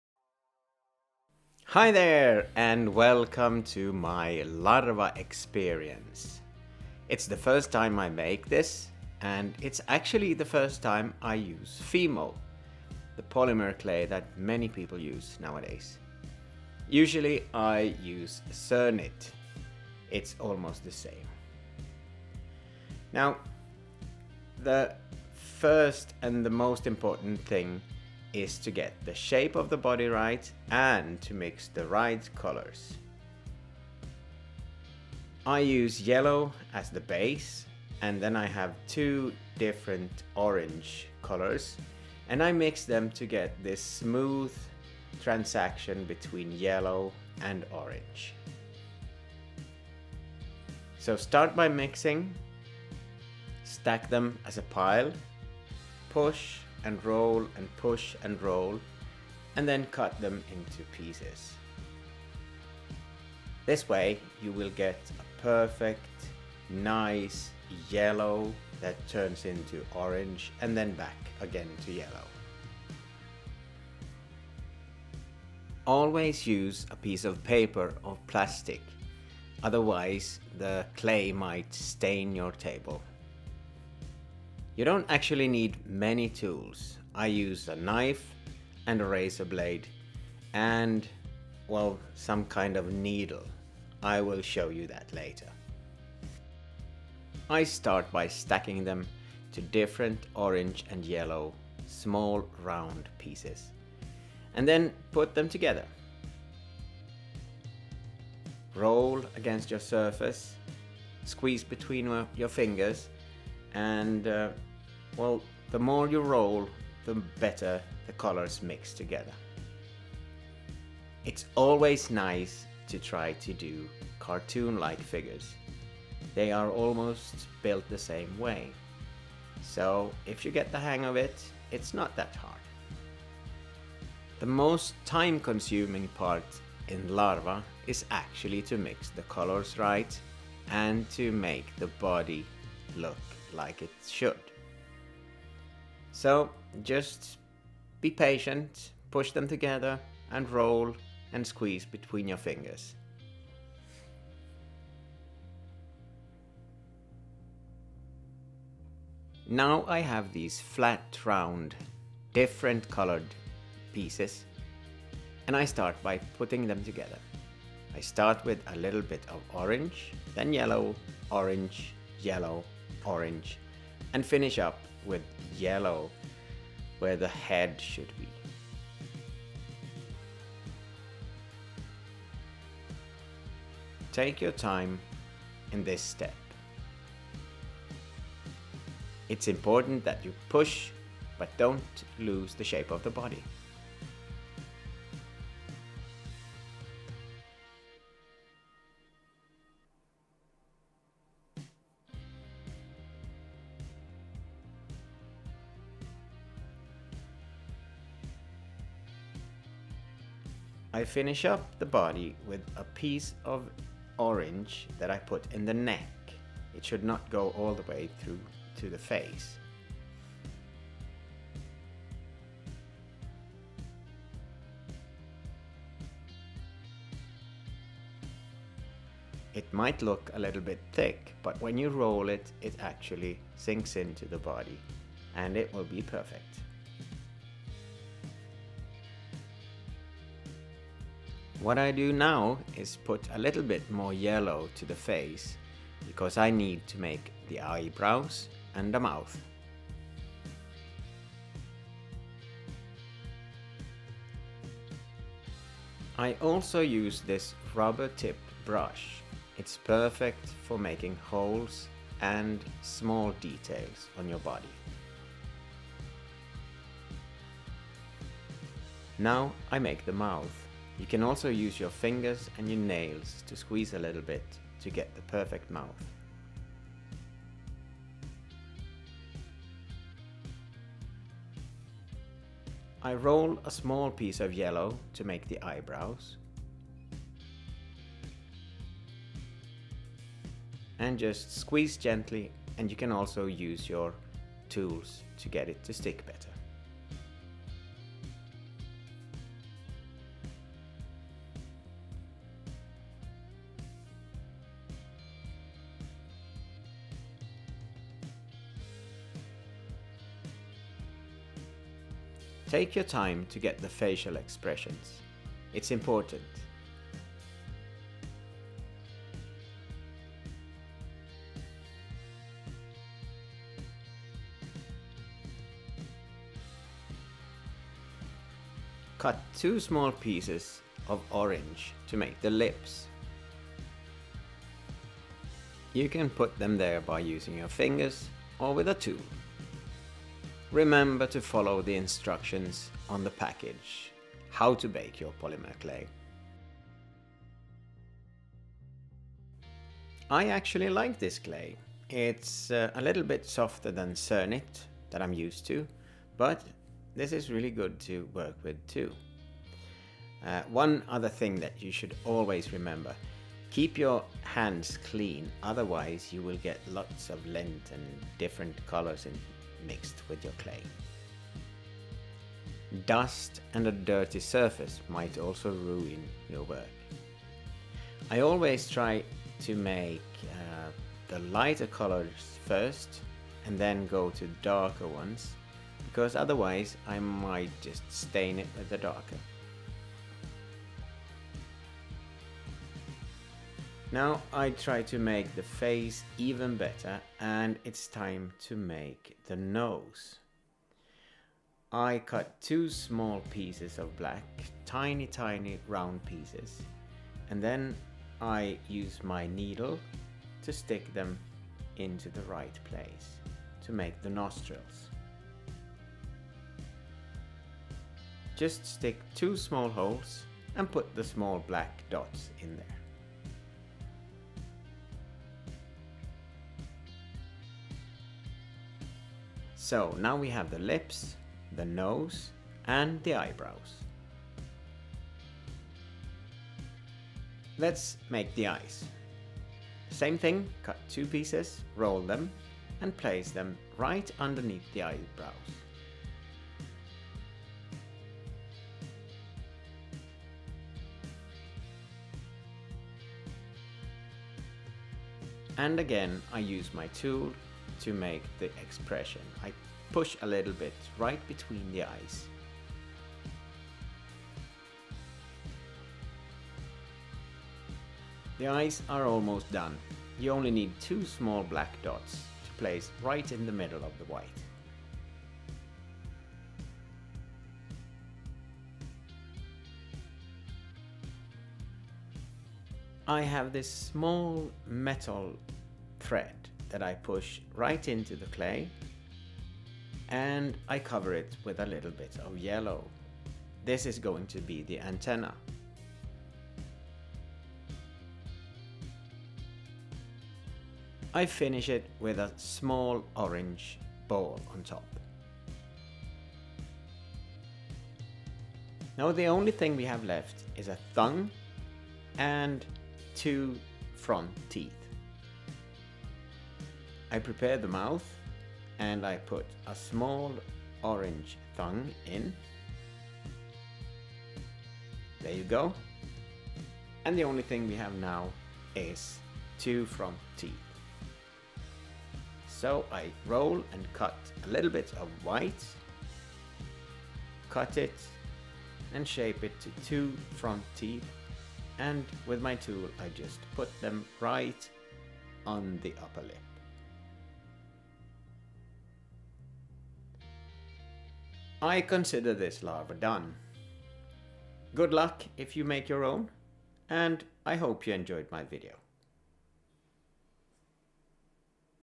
How to make Larva in Polymer clay. Step by step tutorial.
I try Fimo for the first time and make Yellow Larva also for the first time.
This is a tutorial on how to sculpt your own Larva, step- by- step in Fimo, Cernit or other polymer clay.
DIY cartoons and figures in polymer clay are fun for the whole family.